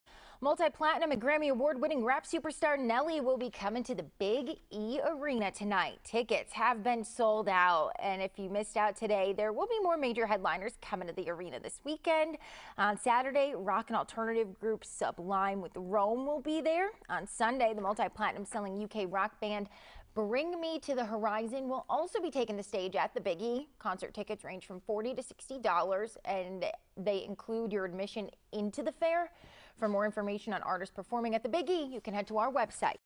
SOLD OUT: Nelly performing at The Big E
WEST SPRINGFIELD, Mass. (WWLP) - Multi-platinum and Grammy award-winning Rap superstar, Nelly will be coming to The Big E Arena Friday night.